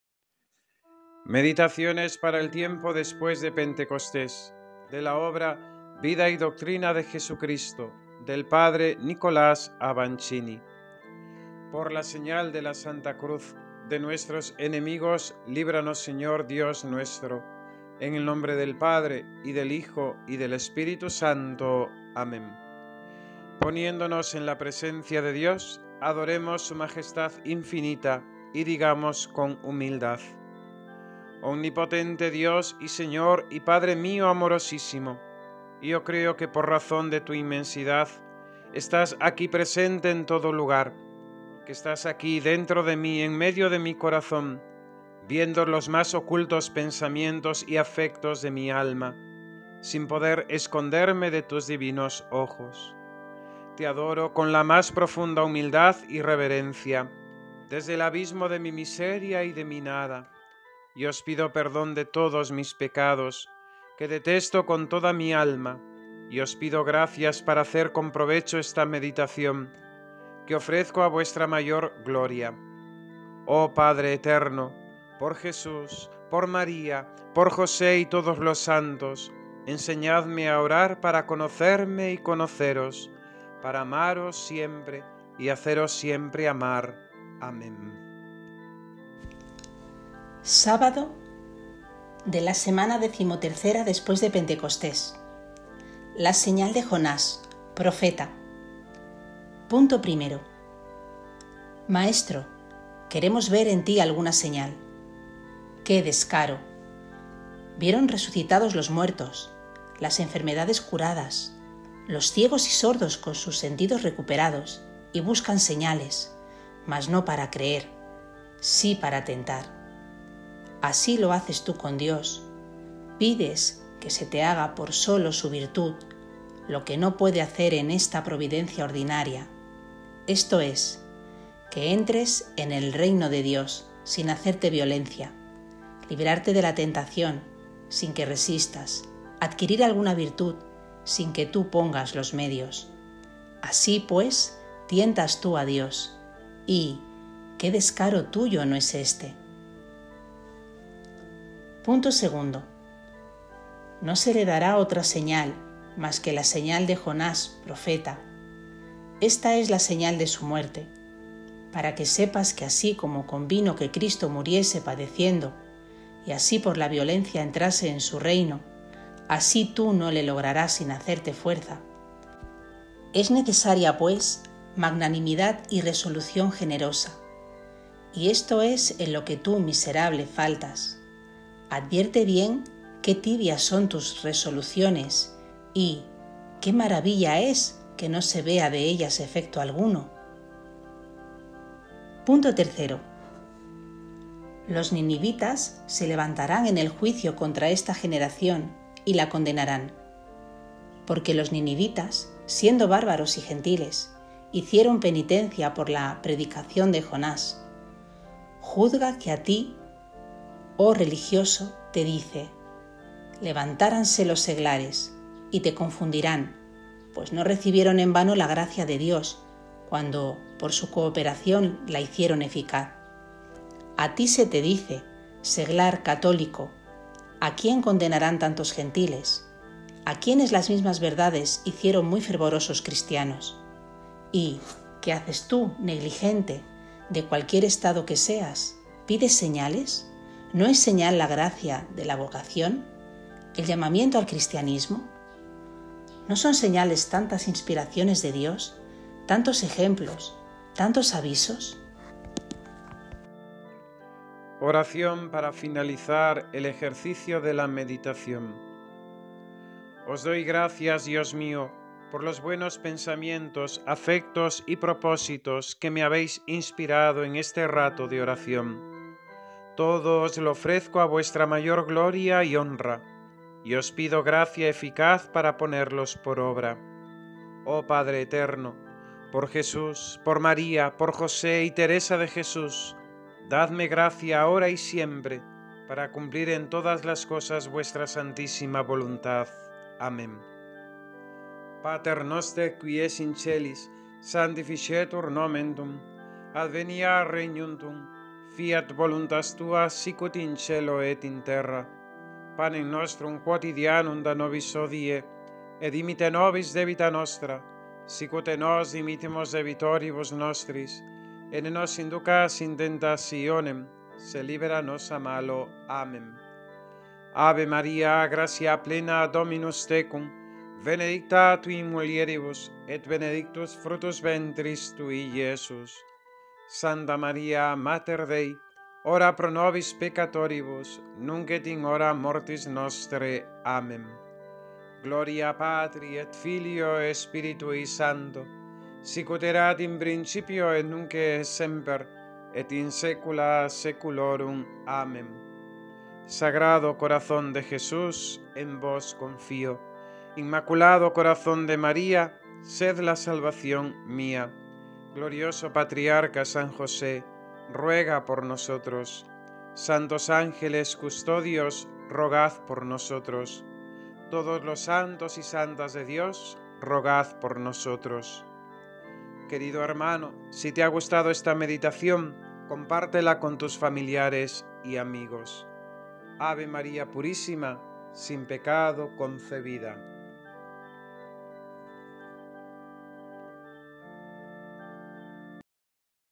LA SEÑAL DE JONÁS PROFETA. Sábado de la XIII semana después de Pentecostés
Ofrecemos este extracto “MEDITACIONES PARA EL TIEMPO DESPUÉS DE PENTECOSTÉS” tomado de la obra “Vida y doctrina de Jesucristo sacada de los cuatro evangelistas y distribuida en materia de meditación para todos los días”  del P. Nicolás Avancini, de la Compañía de Jesús, compuesta en latín, y traducida al castellano por el P. Diego Salgado.
Espero que sea una ayuda para vivir y profundizar más en los misterios de nuestra fe católica a través de la meditación del Santo Evangelio.
Al inicio y final, se recita la oración de san Enrique de Ossó propuesta para el ejercicio de la meditación.
Nuestro agradecimiento a la Hna. Patricia, ermitaña, por la corrección del texto y a Ana Rivero por la grabación de las meditaciones.